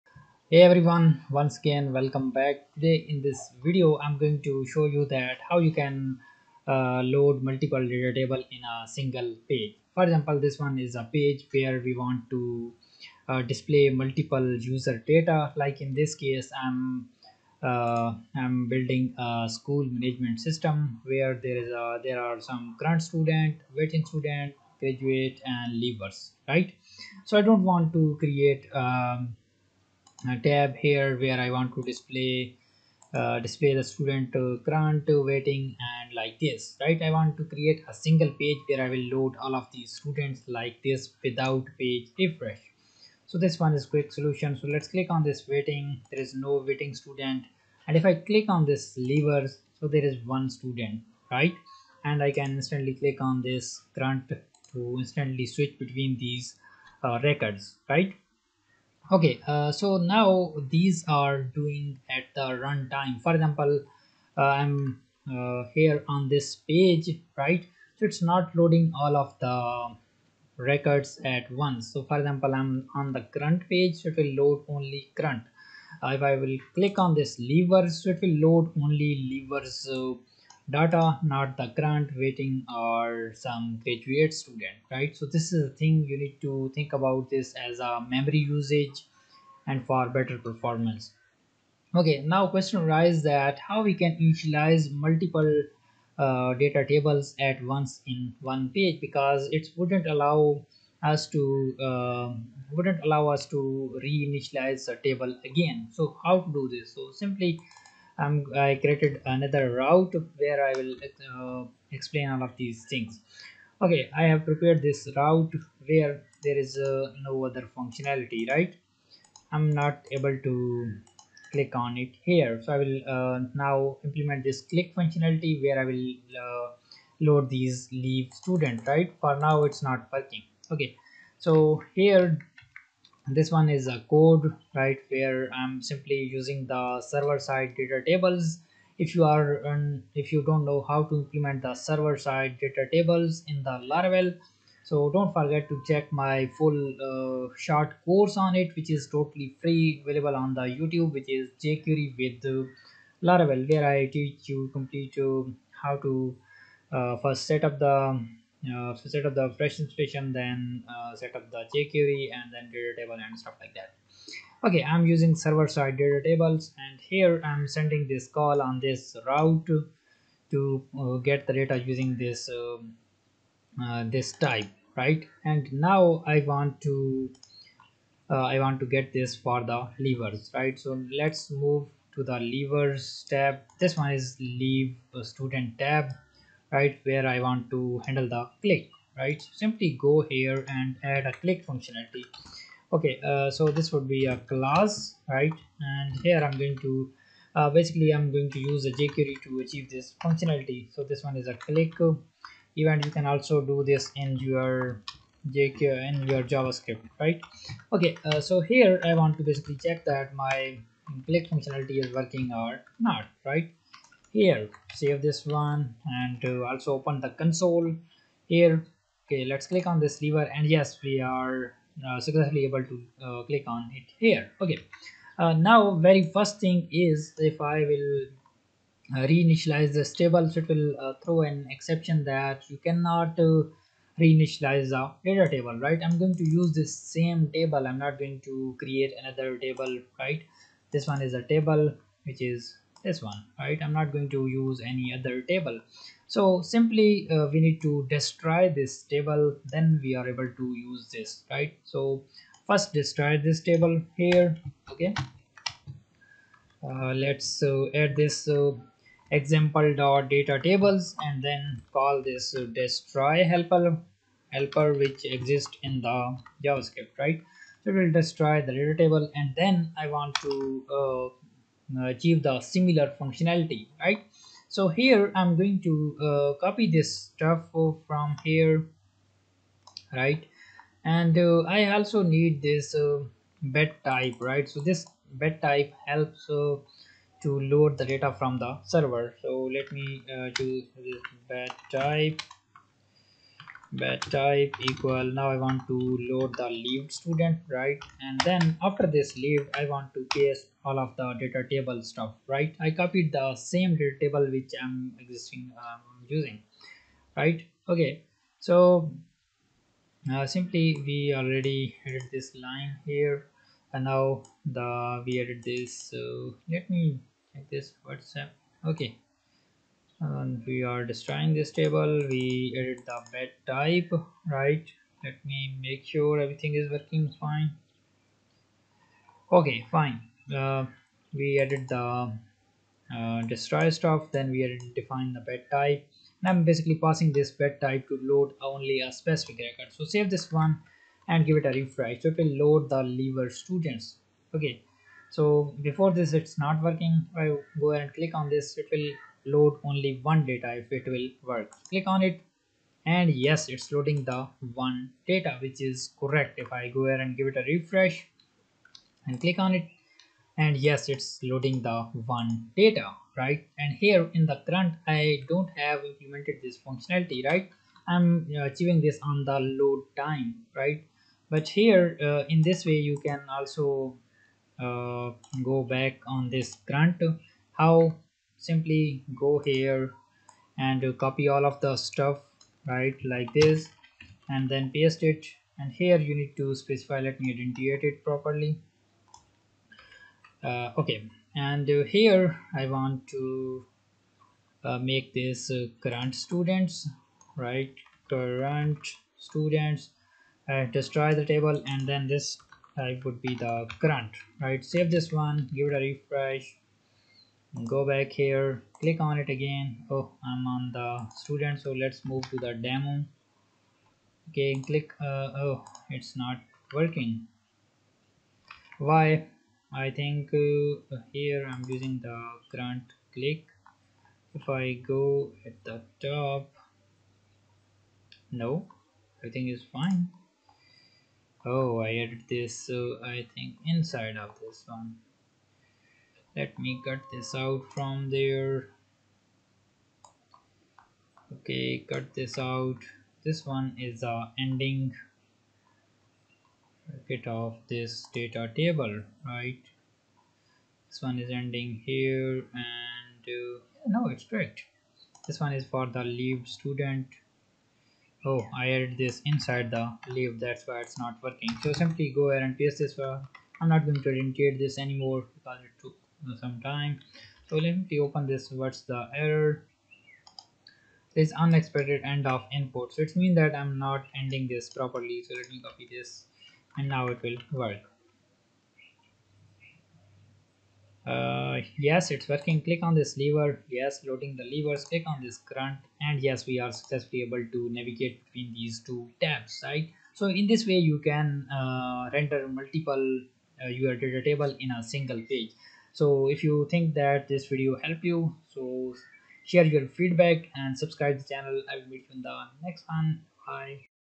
How to Display Multiple DataTables on a Single Page in Laravel
In this tutorial, you will learn how to display multiple DataTables on a single page using Laravel. DataTables is a powerful plugin that allows you to add advanced features such as sorting, searching, and pagination to your HTML tables. However, it can be challenging to display multiple DataTables on the same page, especially if they are loaded asynchronously.

We will start by creating a new Laravel project and installing the DataTables plugin. Then, we will create multiple tables in our view file and initialize them as DataTables using jQuery. We will also demonstrate how to load data into the tables using AJAX requests.

By the end of this tutorial, you will have a good understanding of how to display multiple DataTables on a single page in Laravel and how to load data into them dynamically. So, if you're looking to enhance your HTML tables with advanced features, this tutorial is for you!